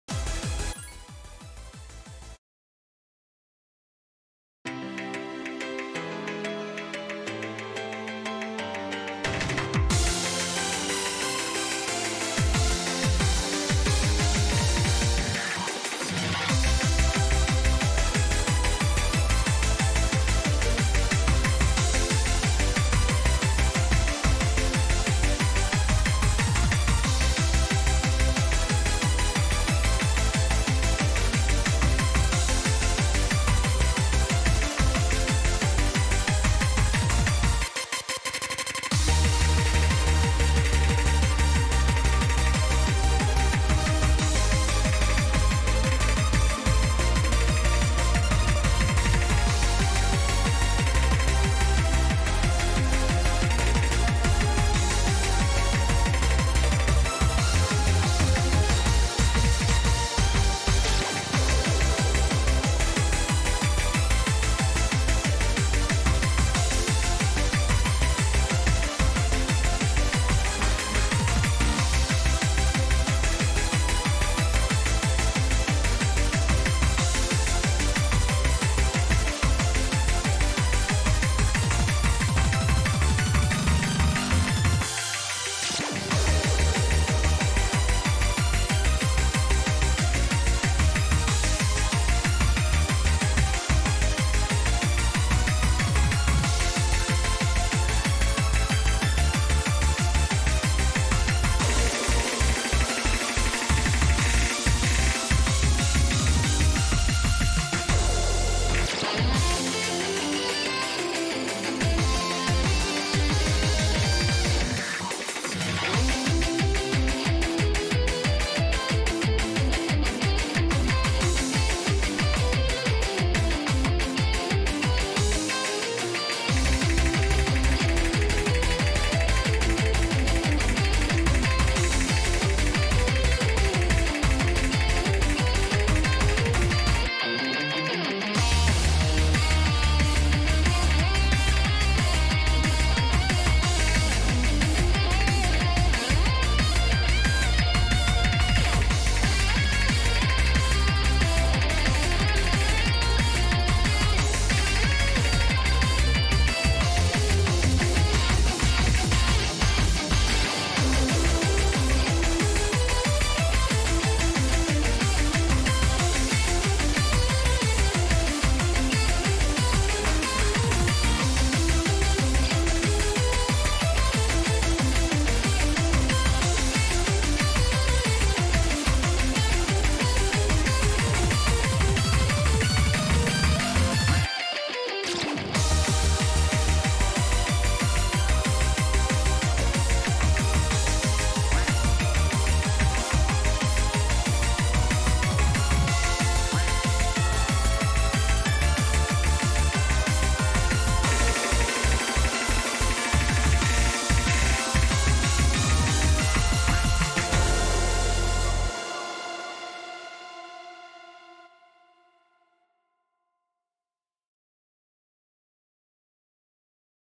quaver from march
first crescendo pass but it was back in March and haven't tried since